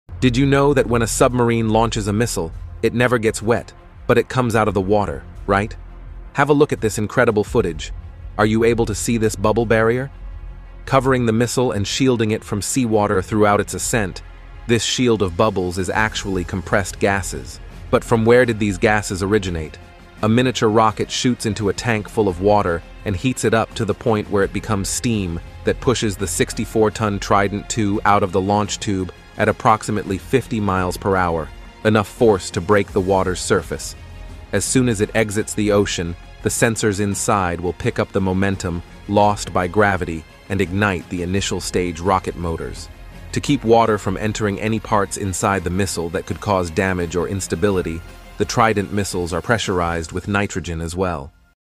Why The Missile Never Gets Wet?
Did you know that when a submarine launches a ballistic missile, the missile never gets wet, but it comes out of the water, right? So what is this phenomenon?